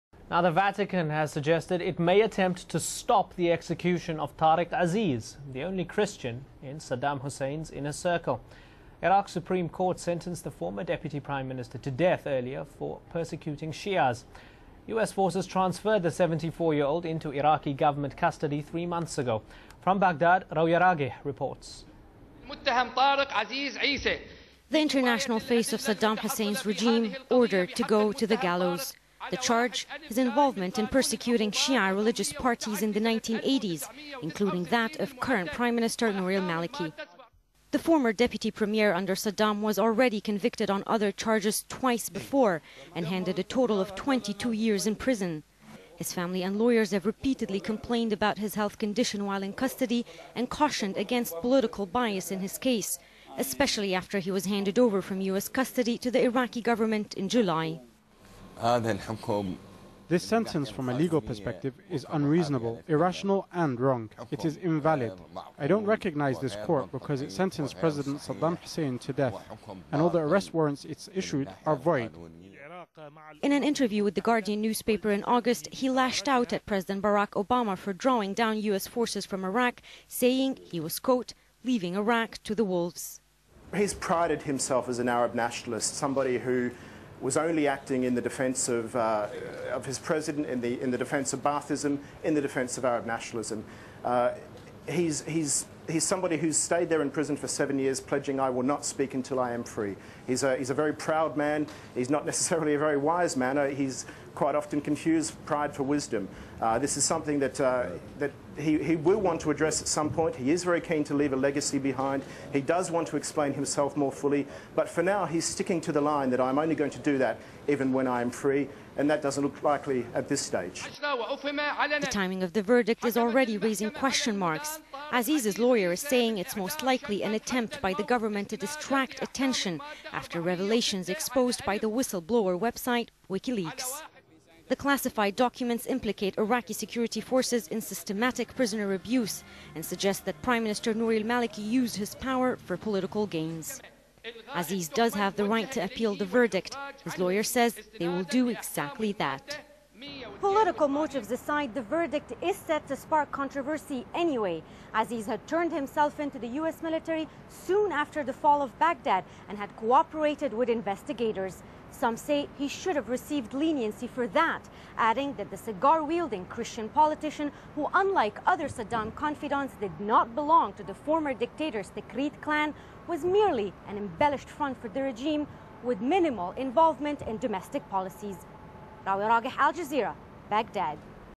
Vatican against execution of ex Iraqi FM Tariq Aziz
Vativcan intend to prevent Tariq Aziz   to be  executed . The Iraqi court sentenced Tareq Aziz and two other high ranking Iraqi closes to Sadam Huseyn on Tuesday October 26 .Tariq Aziz was the unique Christian cl;ose to the inner circle of  Saddam Huseyn .